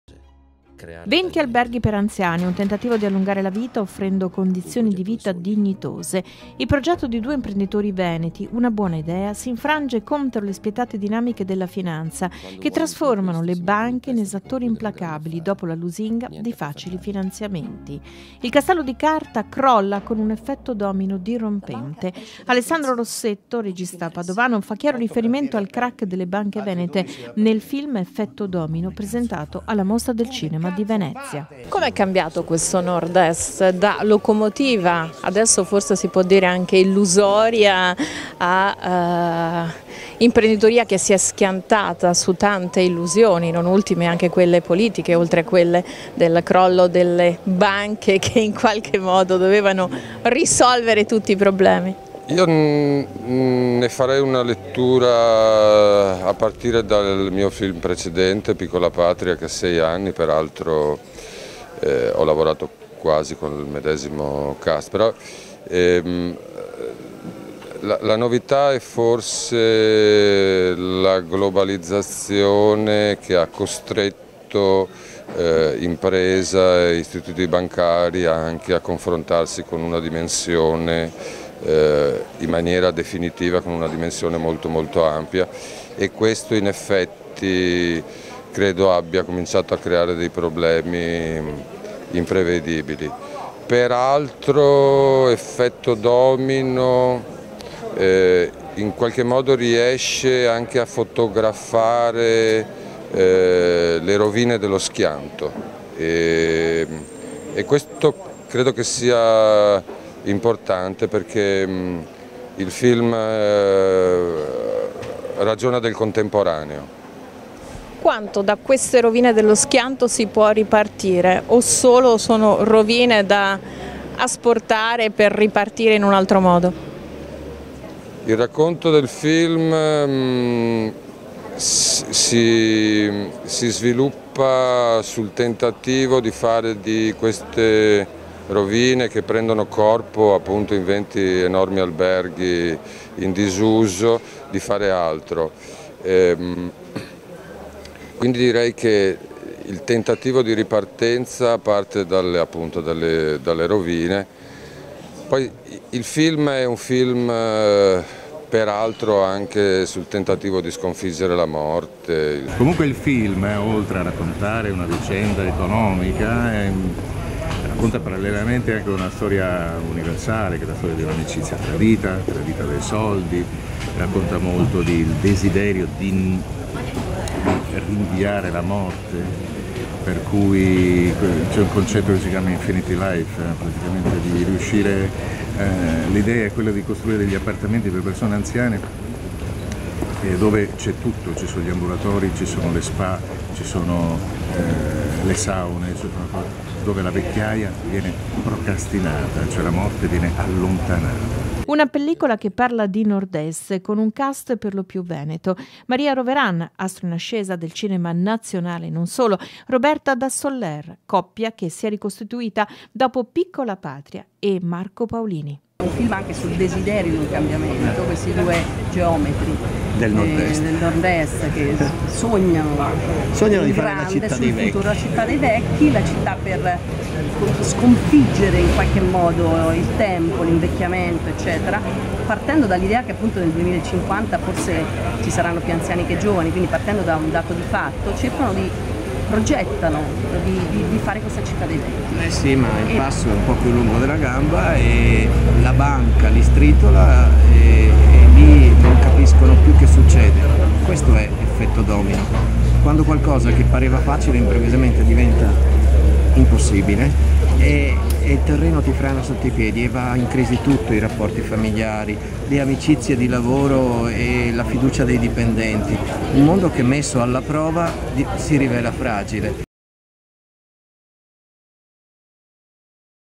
EFFETTO DOMINO
Alla Mostra del Cinema di Venezia, presentato un film veneto, che analizza le conseguenze della crisi per il sistema produttivo e sociale del Nord-Est